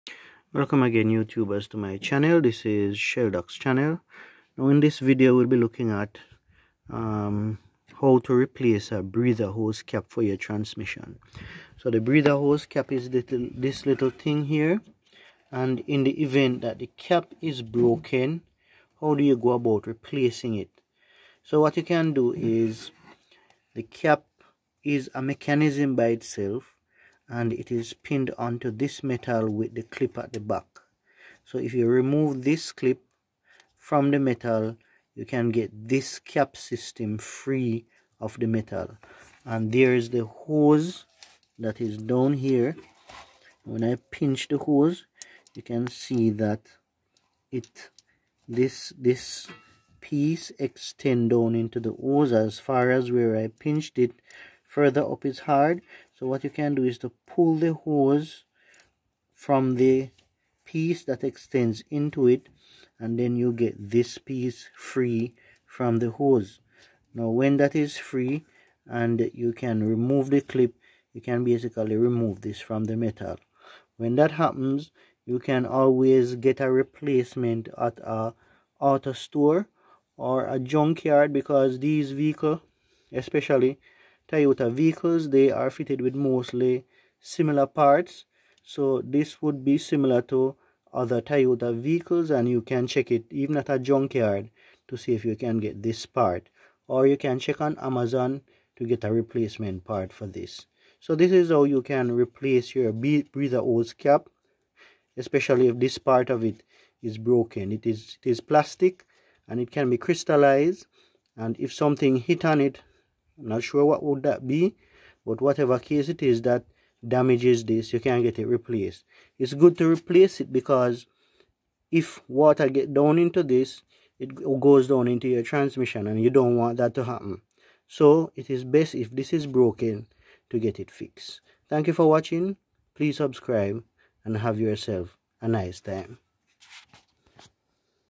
How To Replace Your Transmission Breather Hose Cap
In this video a simple way of replacing your transmission breather hose cam is demonstrated. This breather hose cap is important to the transmission because it allows any pressure built up in the transmission to be vented. A damaged breather hose cap can be displaced causing the hole in the hose to be exposed and allowing water to enter your transmission. Water mixing with transmission fluid will damage your transmission.